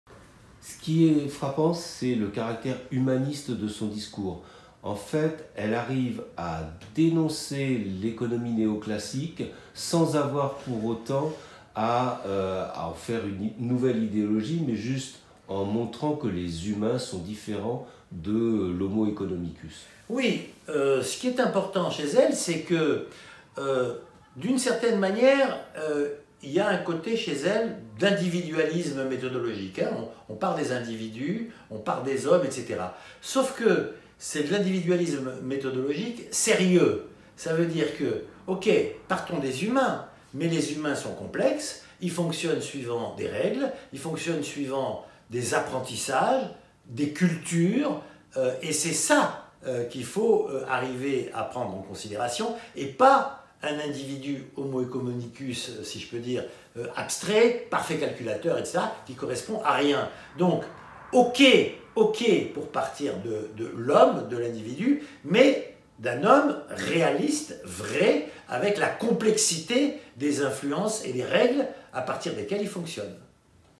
Elinor Ostrom : Penser à partir des humains
Benjamin Coriat a écrit la préface du livre « Discours de Stockholm en réception du Nobel d'économie de 2009» . Il montre ici combien la méthode Ostrom part des humains réels et non de l'abstraction de l'homo oeconomicus de l'école néoclassique.

Discours de Stockholm, en réception du Nobel d'économie 2009
Elinor Ostrom
Préface de Benjamin Coriat («Ecouter Ostrom»)
Collection Interventions, 3
ISBN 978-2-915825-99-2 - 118 p. - janvier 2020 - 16 €